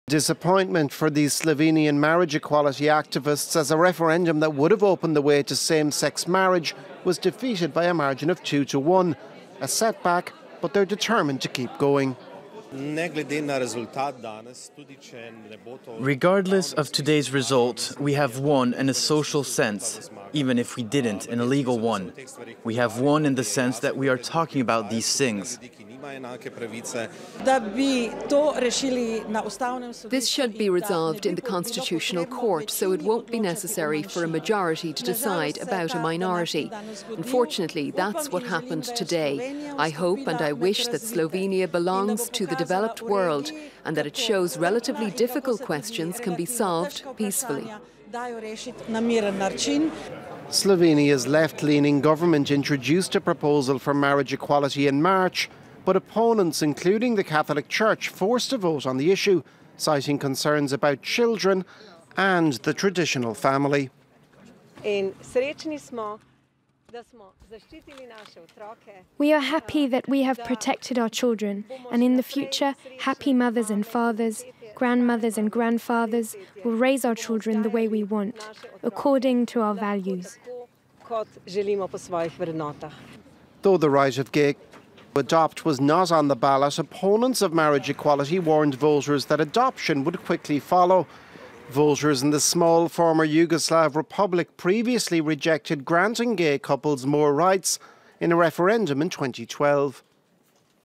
Slovenians reject same-sex marriage by large margin in referendum
Slovenians rejected a same-sex marriage law by a large margin in a referendum on Sunday, according to preliminary referendum results.

The results released Sunday by authorities show 63 percent voted against a bill that defines marriage as a union of two adults, while 37 percent were in favor.